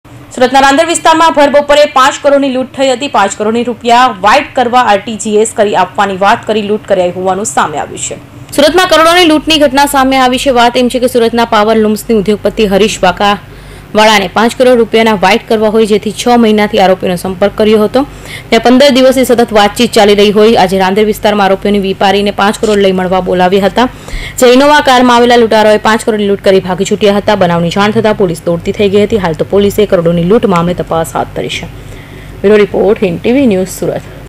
સુરતઃ રાંદેર રોડ વિસ્તારમાં 5 કરોડ રૂપિયાની લૂંટ
લૂંટની ઘટના બાદ વચેટીયાના સીસીટીવી ફુટેજ સામે આવ્યા
પોલીસે કરોડોની લુંટ મામલે તપાસ હાથ ધરી